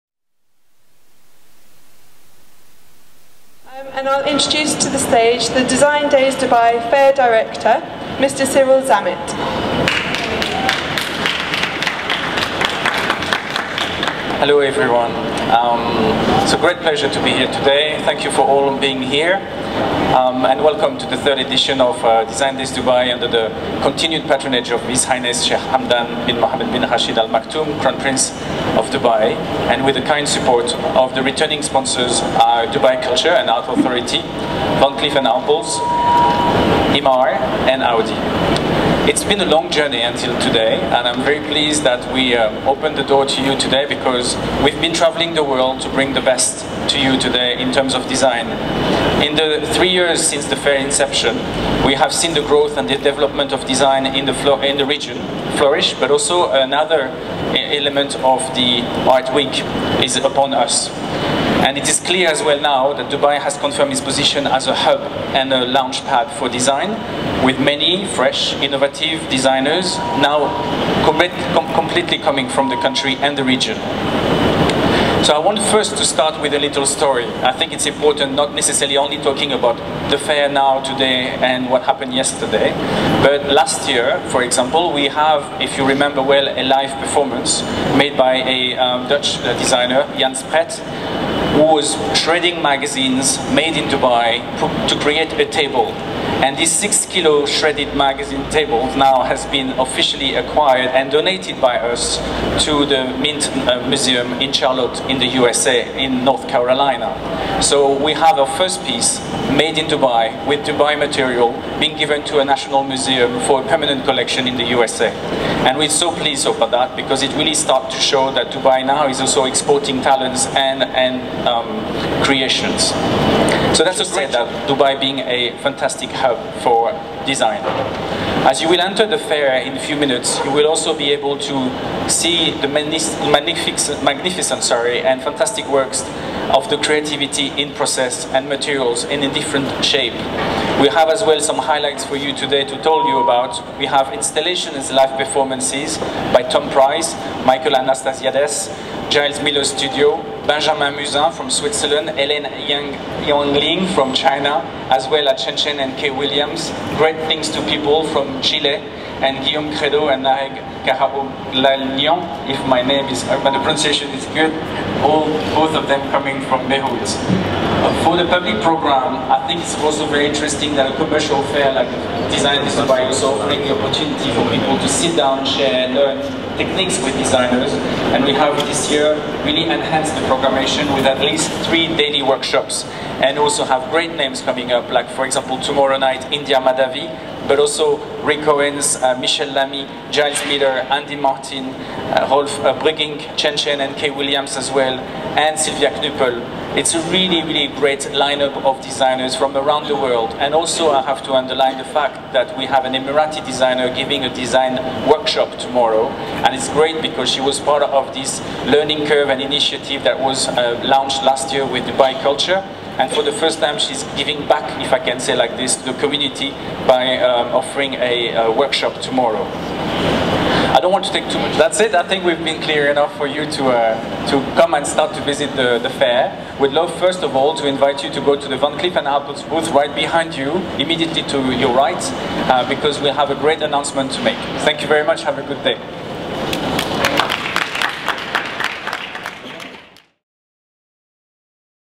Design Days Dubai 2014 - Press Conference
Footage of the 3rd annual Design Days Dubai Press Conference held on March 16th 2014.